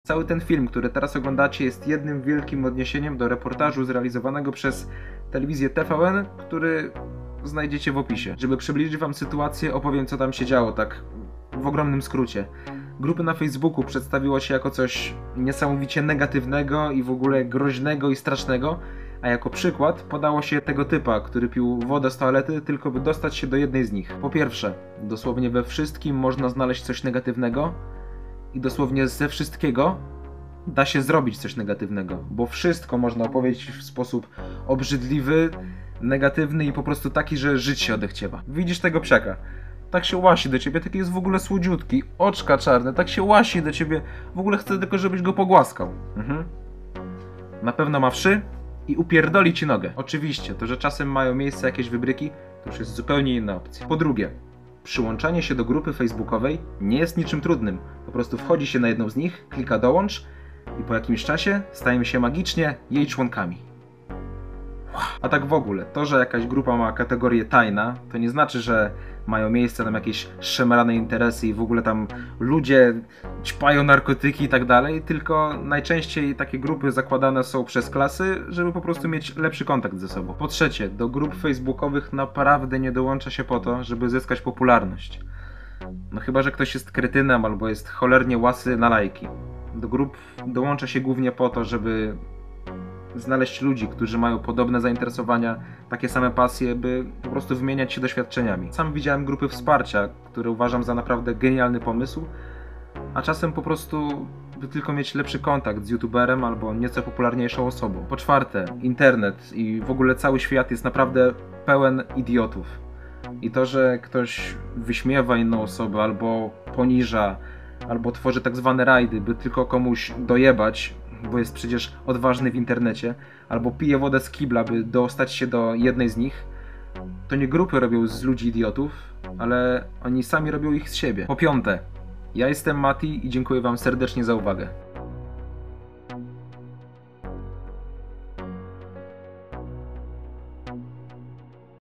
Niebezpieczne grupy na Facebooku
- Co się dzieje u mnie?

Niebezpieczeństwo, strach, tragedia, śmierć, obrzydliwości, bałagan, chaos, piekło, wymioty, anarchia piekło! Tak.. mowa o grupach na facebooku.. według TVN'u! :D